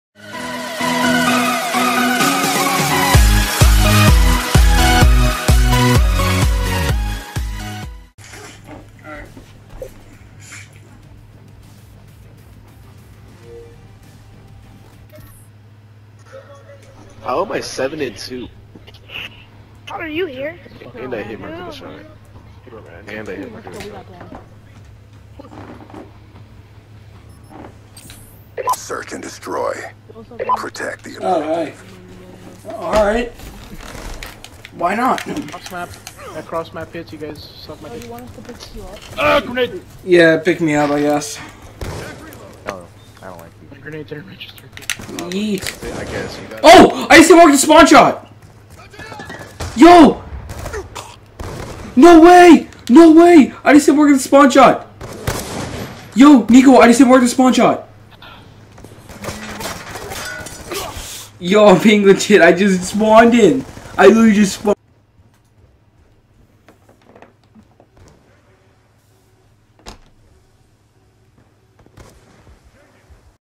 Crazy HM ForThe Dare RC
I Joined My Friends 6 Cause They Lost One Person And I Hit Markered The Spawn Shot First Try :( Like And Sub For The Pain